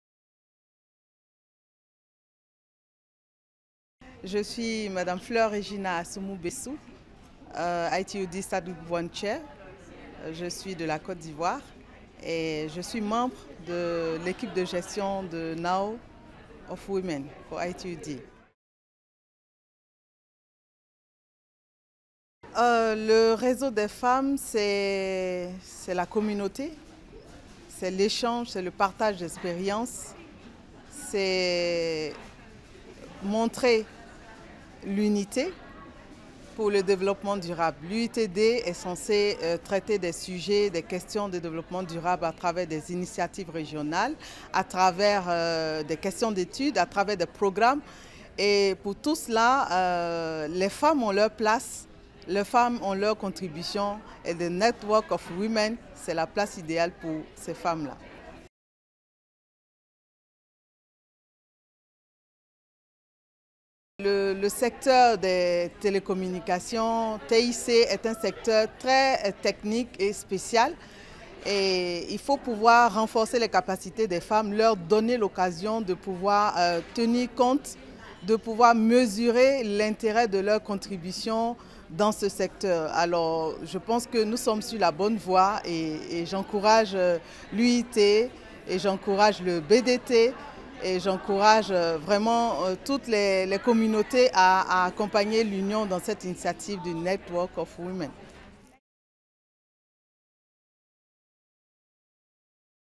Network of Women - Women’s Breakfast at ITUWTDC: Interview with Fleur Regina Assoumou-Bessou
The Network of Women (NoW) in the Telecommunication Development Sector (ITU-D) hosted a Women's Breakfast during the World Telecommunication Development Conference (ITUWTDC). The goal of this breakfast conversation was to collate ideas on promoting gender equality in the field of telecommunications/ICTs through building on existing efforts including bringing to life, the gender-related WTDC conference resolutions, in particular WTDC RESOLUTION 55 (Rev. Buenos Aires, 2017) - Mainstreaming a gender perspective for an inclusive and egalitarian information society.

We asked some of the attendees to share with us their thoughts on what it means to them to be part of the Network of Women as well as their thoughts on how the representation of women in the ICT sector is changing.